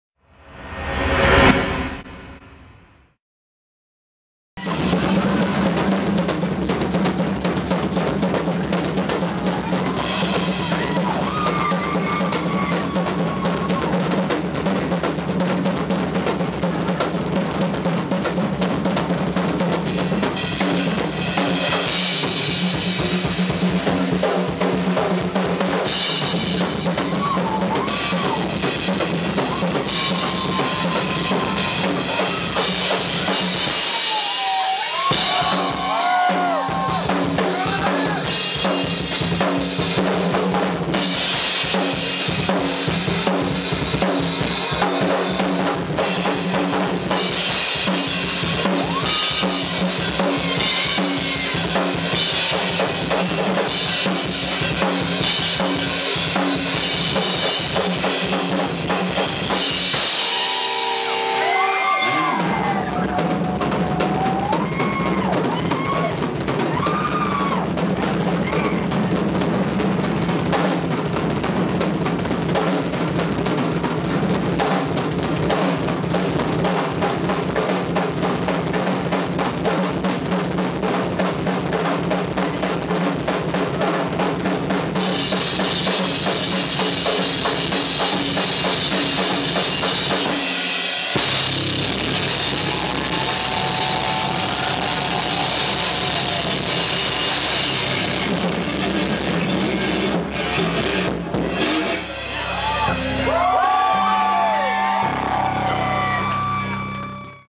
The Rabid Whole Drum Solo.mov
Drum solo by JJ Tartaglia of The Rabid Whole at the Bovine Sex Club, Oct 11, 2012 INDIE WEEK Toronto